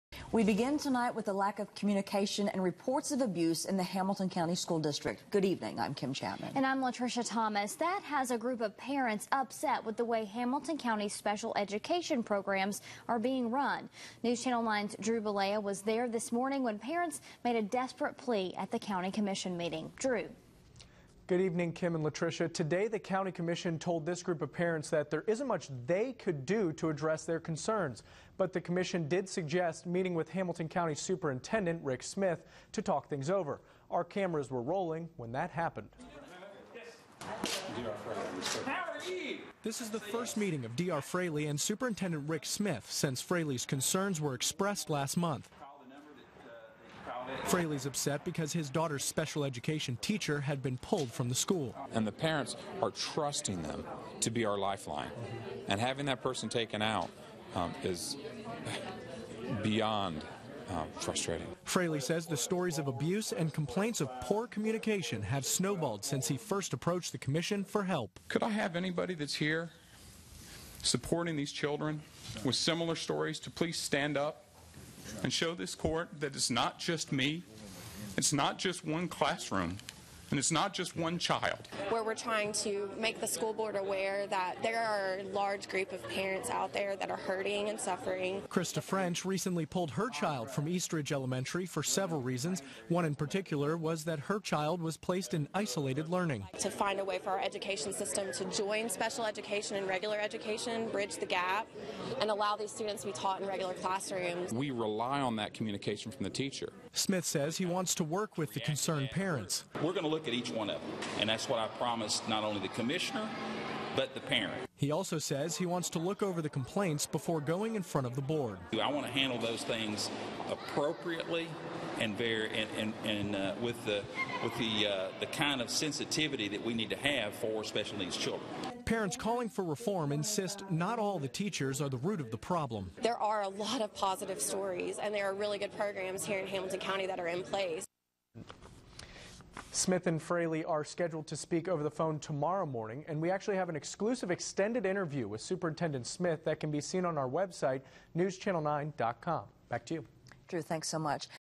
Parents Express Concerns Over Special Needs Issues
By Drew Bollea  A lack of communication and reports of abuse in the Hamilton County school district is what drove more than a dozen parents to the Hamilton County Commission meting Wednesday morning.A group of parents are upset with the way Hamilton County special education programs are being run. Commission Chairman Larry Henry told D.R. Fraley, who represented the group of parents, that the commission could only be an ear to listen and couldn't act on the complaints. He suggested that Mr. Fraley take his concerns to the school board, and specifically, Supt. Rick Smith. Outside of a Parent Teacher Association meeting, Fraley and Supt. Rick Smith had an unplanned meeting. Fraley gave Smith a folder filled with both positive and negative messages about the special education program. The two then parted ways. Fraley's upset because his daughter's special education teacher had been pulled from the school."The parents are trusting them to be our lifeline," said Fraley after today's commission meeting, "and having that person taken out is beyond frustrating."Fraley says the stories of abuse, and complaints of poor communication, have snowballed since he first approached the commission for help."Could I have anybody that's here supporting these children with similar stories to please stand up," said Fraley in front of the commission. He continued as more than a dozen parents rose from their seats, "this shows its not just me, its not just one classroom, its not just one child."Christa French recently pulled her child from East Ridge Elementary  for several reasons, one in particular, her child was placed in isolated learning."I'd like the board to find a way for our education system to join special education and regular education and bridge the gap," said French, "and allow these students to be taught in regular classrooms."Lack of communication between student and teacher was another concern from Fraley."We are dealing with children who struggle with communication," said Fraley, "we rely on that communication from the teacher."Smith says he wants to work with the concerned parents. "We're going to look at each one of them," said Smith talking about the complaints, "that's what I promised not only the commissioner, but the parent."Smith says he wants to look over the complaints before going in front of the board."I want to handle those things appropriately and with the kind of sensitivity we need to have for special needs children," said Smith.Parents desperate for help say while they are bringing the negatives to light, its not all bad news."There are a lot of positive stories," said French, "and there are really good programs in Hamilton County in place."Smith and Fraley are scheduled to speak over the phone Thursday morning.Supt. Smith spoke only to News Channel 9 about his experience as a parent of a child with special needs. That interview can be seen HERE